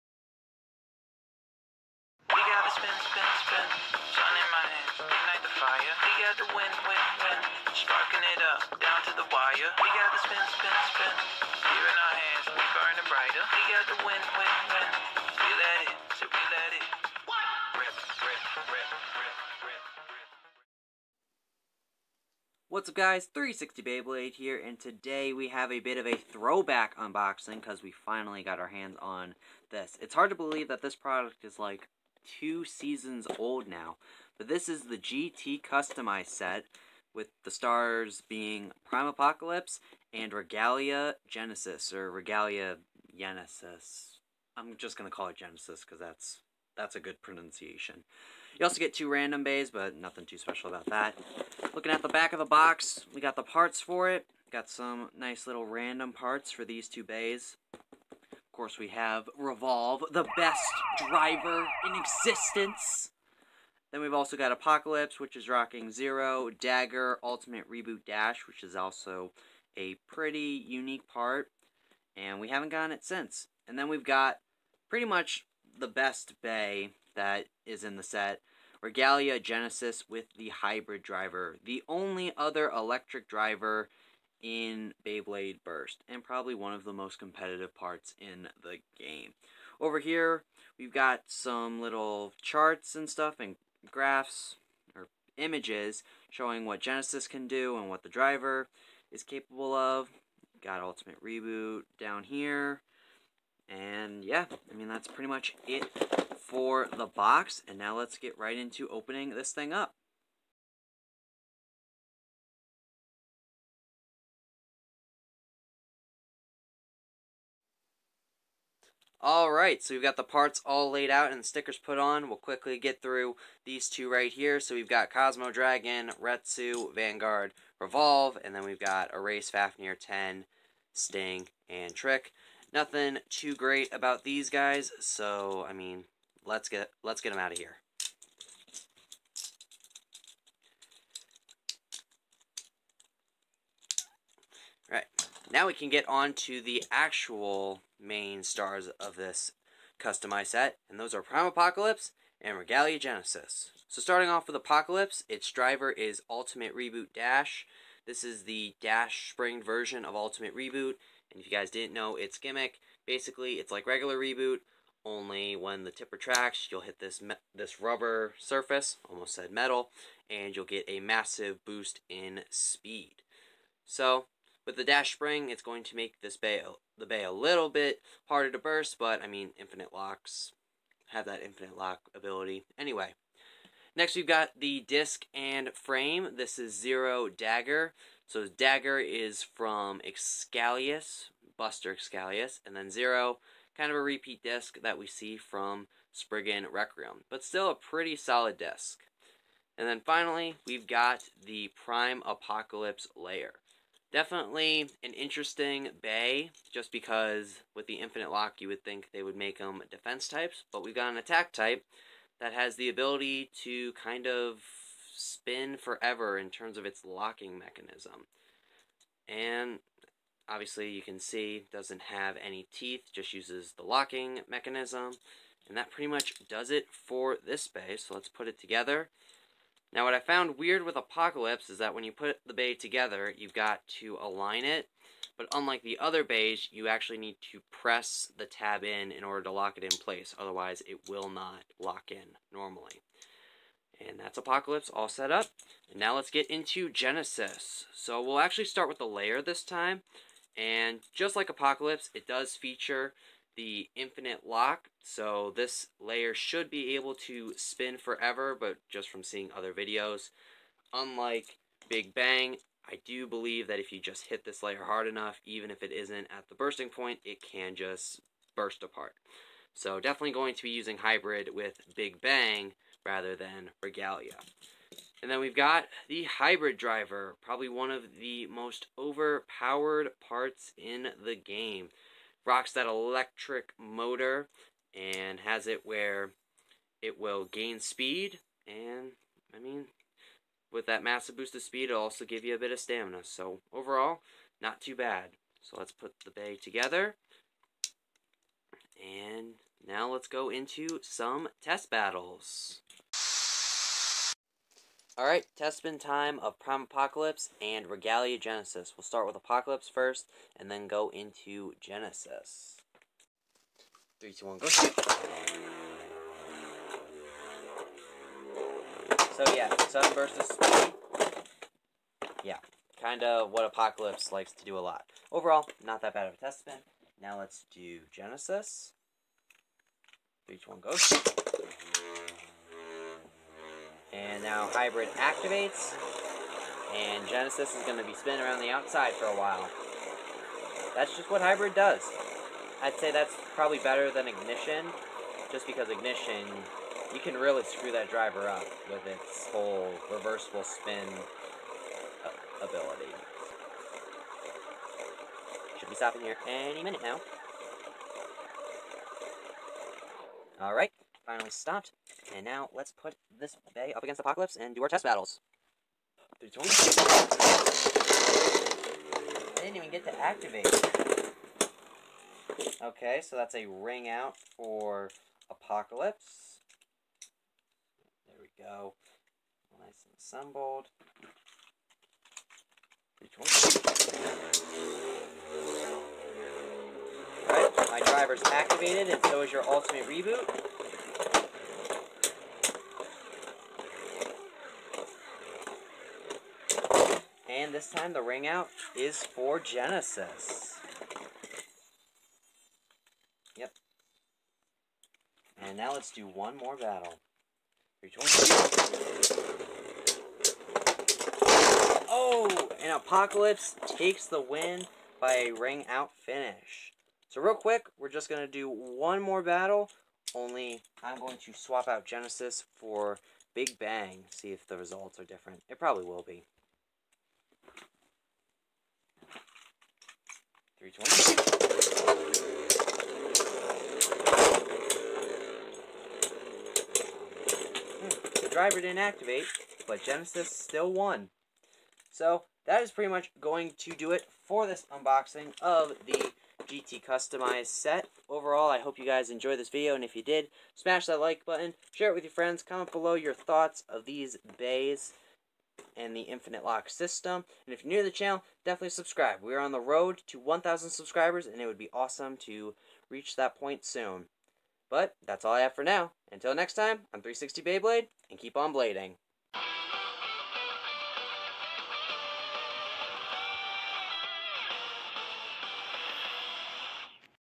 The Greatest Ability Ever. Beyblade burst GT Customization set unboxing & Review.
Quick disclaimer, this video along with all past and future videos are designed for ages 13+.
What's up everyone 360 Beyblade here finally unboxing the GT customize set. This set is probably one of the best in my opinion and definitely a difficult one to get in 2021.